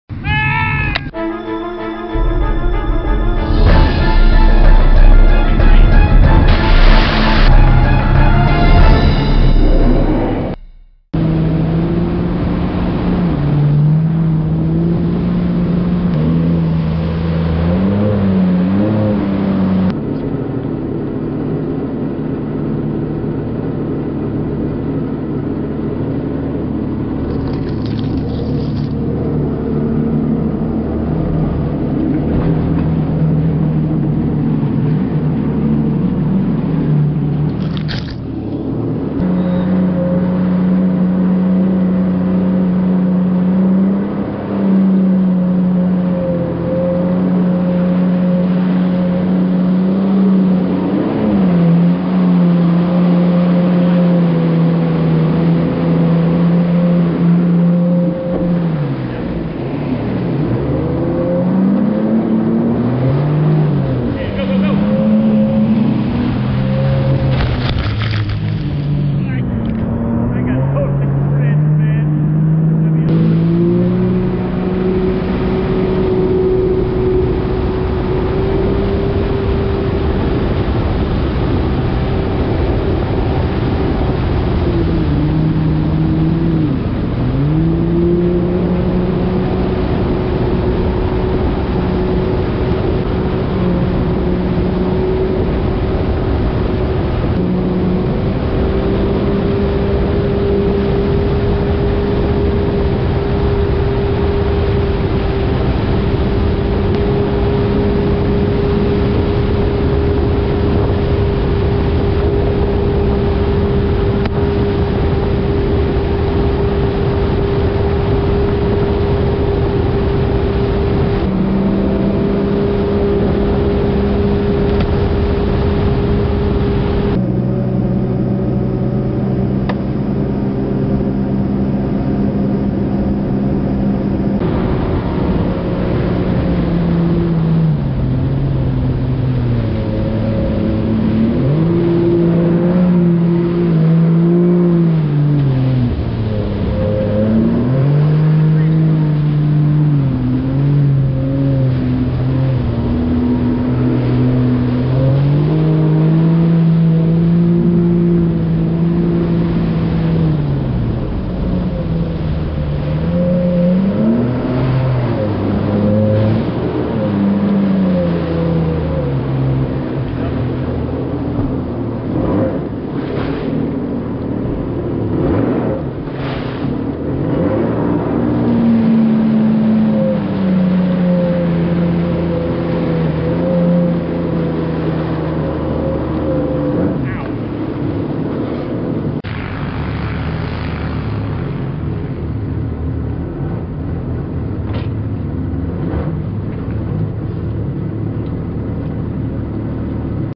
Jet ski rusty drainage pipe, almost hit cow, and other foolishness
Jet ski rusty drainage pipe, almost hit a cow, and other foolishness
jet ski fails, Florida man